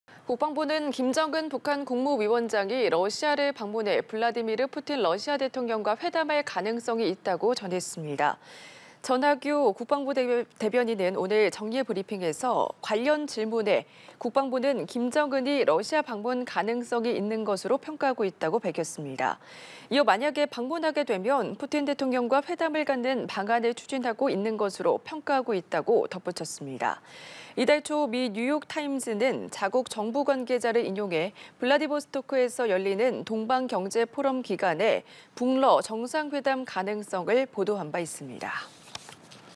국방부 "김정은, 방러해 푸틴과 회담 가능성 있어" / 연합뉴스TV (YonhapnewsTV)
국방부 "김정은, 방러해 푸틴과 회담 가능성 있어"

국방부는 김정은 북한 국무위원장이 러시아를 방문해 블라디미르 푸틴 러시아 대통령과 회담할 가능성이 있다고 전했습니다.

전하규 국방부 대변인은 오늘(11일) 정례 브리핑에서 관련 질문에 "국방부는 김정은이 러시아 방문 가능성이 있는 것으로 평가하고 있다"고 밝혔습니다.

이어 "만약에 방문하게 되면 푸틴 대통령과 회담을 갖는 방안을 추진하고 있는 것으로 평가하고 있다"고 덧붙였습니다.

이달 초 미 뉴욕타임스는 자국 정부 관계자를 인용해 블라디보스토크에서 열리는 동방경제포럼 기간에 북러 정상회담 가능성을 보도한 바 있습니다.